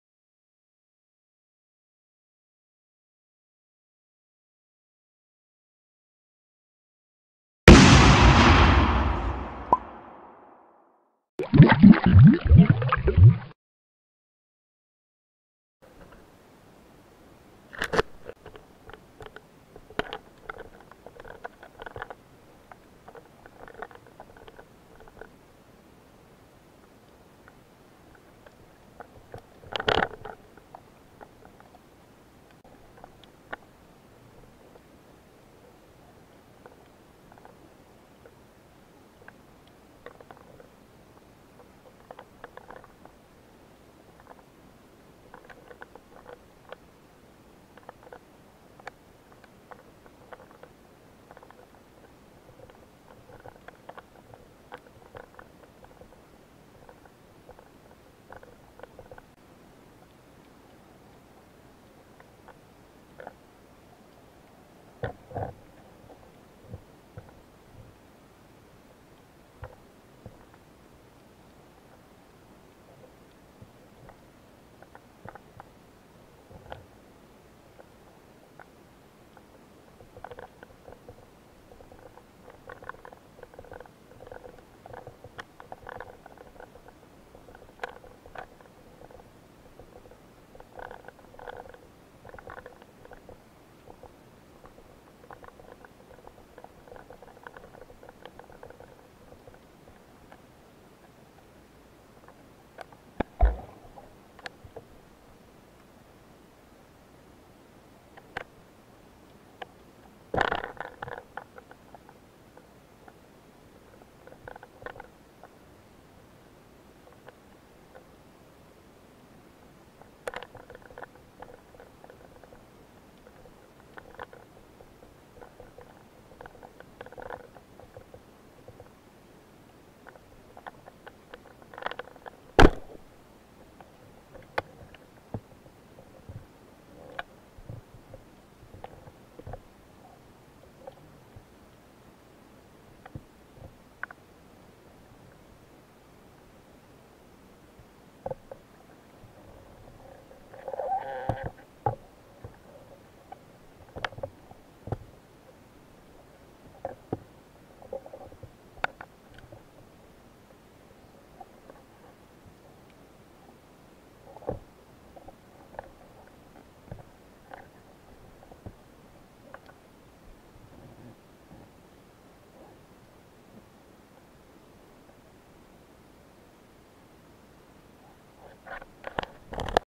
pesca sub , pescata del 31/12/18 , spigola ,AUGURI A TUTTI
buongiornò a tutte le mondò .

AUGURI A TUTTI

mare bellissimo scaduta con onda rabbiosa in alcuni punti a sud , acqua bella considerando ieri con la maestralata a sud , bassa marea

pochi pesci visi uno solo catturato cercato desiderato trovato nel deserto almeno 15 tuffi in un metro di fondo perduti per l onda che mi sdradicava dall appostamento , ho rotto anche una pala .

chi puo vada che merita di provare soprattutto se riuscite a buttarvi con l alta marea

grazie e ciao